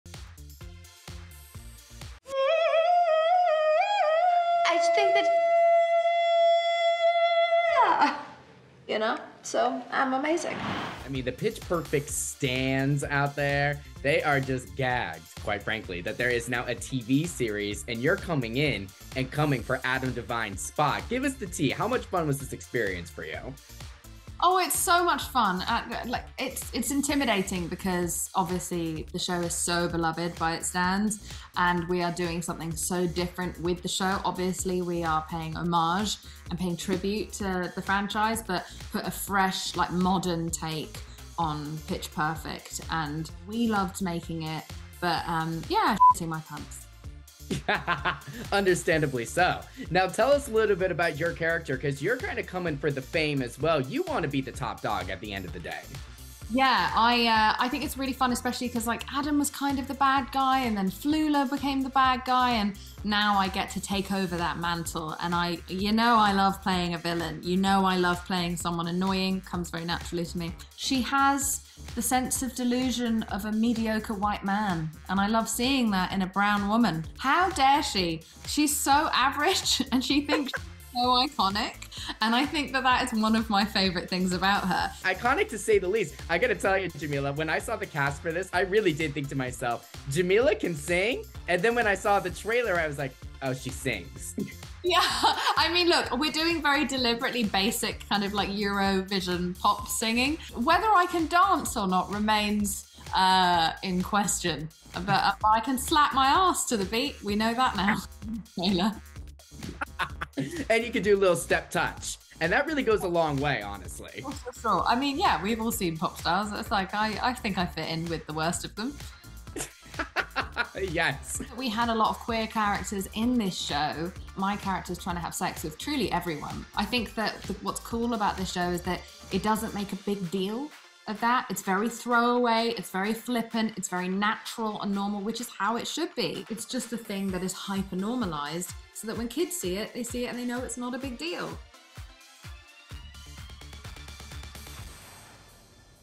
Jameela Jamil Spills on Her "Average" Role in 'Pitch Perfect' Spin-Off
JameelaJamil is one of the new faces to join the PitchPerfect franchise! The star is catching up with our Ricky Cornish on her somewhat iconic role. BumperInBerlin is streaming now on Peacock.